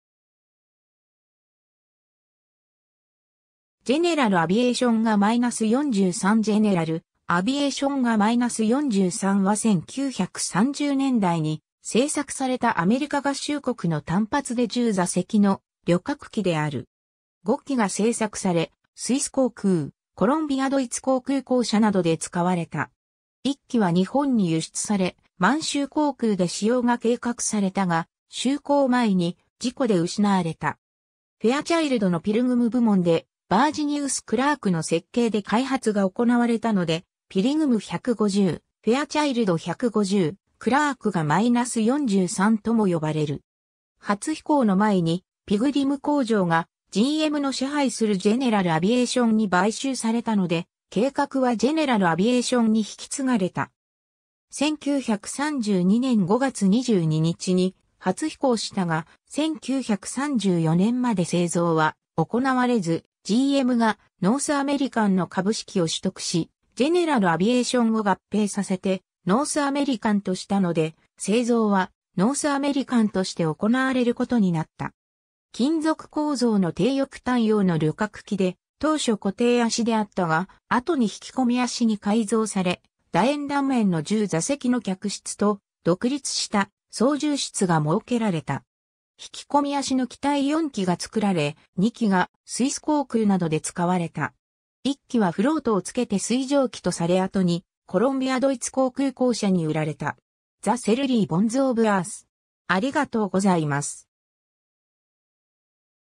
ジェネラル・アヴィエーション GA-43
ジェネラル・アヴィエーション GA-43（モデル） ジェネラル・アヴィエーション GA-43（General Aviation GA-43）は1930年代に製作されたアメリカ合衆国の単発で10座席の旅客機である。
5機が製作され、スイス航空、コロンビア・ドイツ航空公社などで使われた。
1機は日本に輸出され、満州航空で使用が計画されたが就航前に事故で失われた。
 フェアチャイルドのピルグム部門でバージニウス・クラーク（Virginius E. Clark）の設計で開発が行われたので、ピリグム 150、フェアチャイルド 150、クラーク GA-43とも呼ばれる。
初飛行の前に、ピグリム工場がGMの支配するジェネラル・アヴィエーションに買収されたので、計画はジェネラル・アヴィエーションに引き継がれた。
1932年5月22日に初飛行したが、1934年まで製造は行われず、GMがノースアメリカンの株式を取得し、ジェネラル・アヴィエーションを合併させてノースアメリカンとしたので、製造はノースアメリカンとして行われることになった。
金属構造の低翼単葉の旅客機で、当初固定脚であったが、後に引き込み脚に改造され、楕円断面の10座席の客室と、独立した操縦室が設けられた。
引き込み脚の機体4機が作られ、2機がスイス航空などで使われた。
1機はフロートをつけて水上機とされ後にコロンビア・ドイツ航空公社に売られた。
 ^ The Surly Bonds of Earth